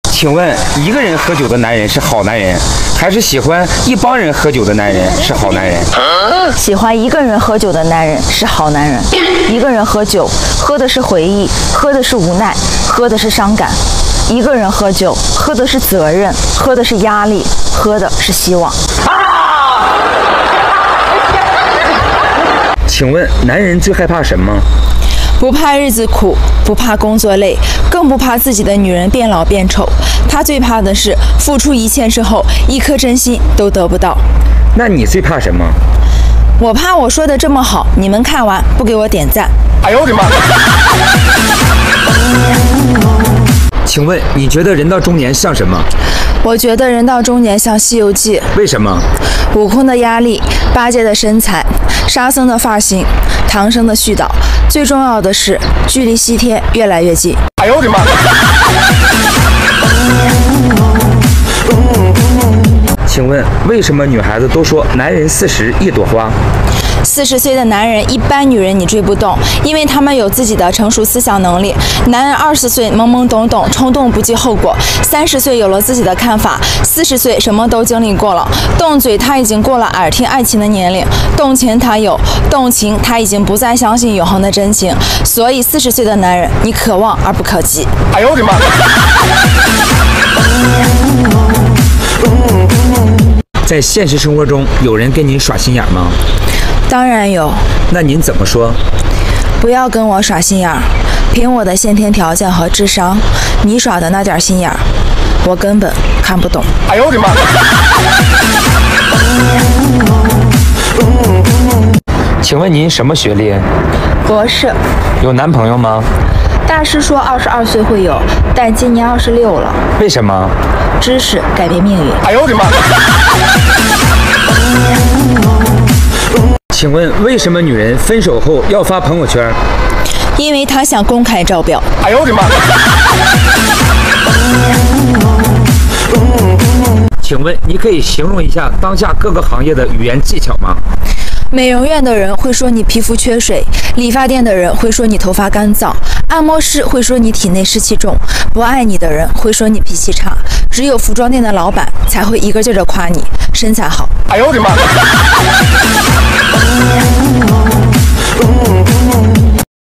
【毛三儿】请问男人最害怕什么？美女幽默精彩回答，句句在理，配音改编
➤请问男人最害怕什么？美女幽默精彩回答，句句在理，配音改编